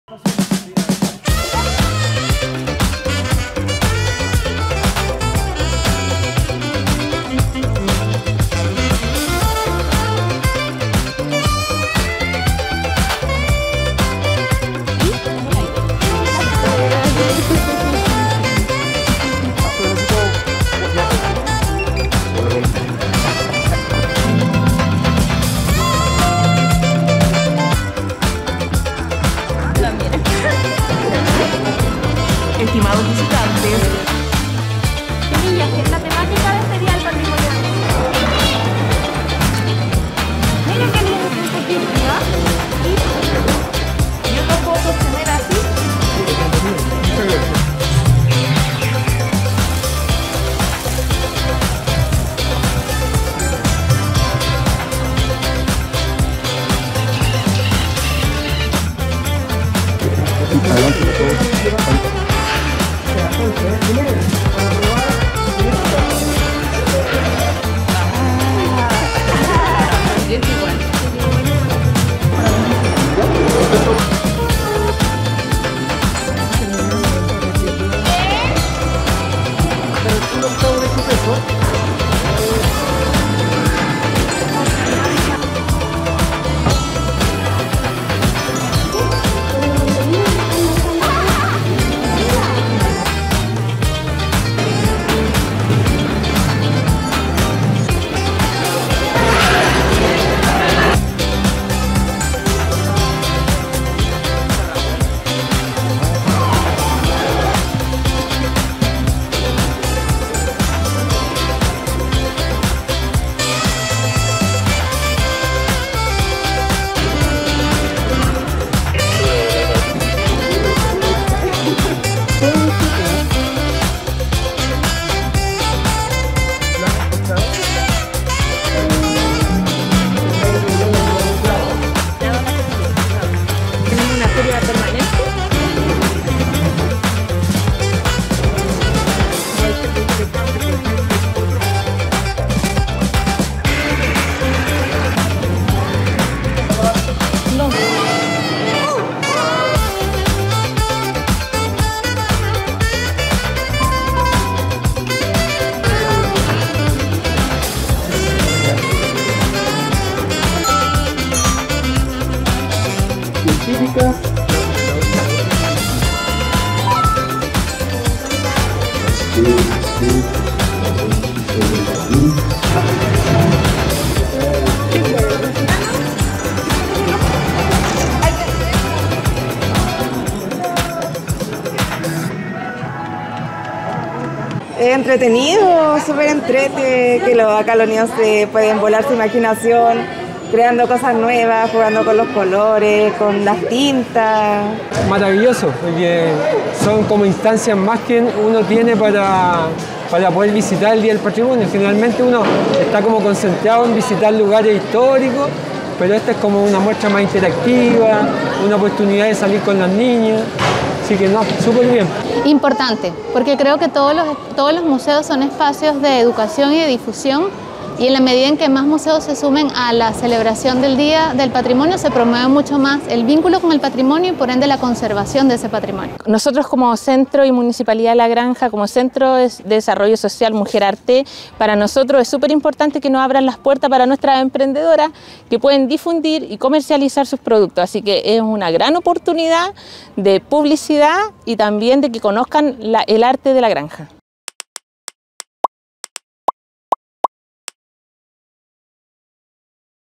Así fue el Día de los Patrimonios en el MIM
Este año por primera vez en su historia el Museo Interactivo Mirador (MIM) fue parte del Día de los Patrimonios en forma presencial, abriendo sus puertas a la ciudadanía en forma gratuita durante el sábado 28 y domingo 29 de mayo; en una jornada que buscó rescatar el ADN artístico del museo, mediante actividades que potencian su esencia y diversidad cultural, donde la creatividad, las artes, las ciencias y los patrimonios se conjugan y articulan.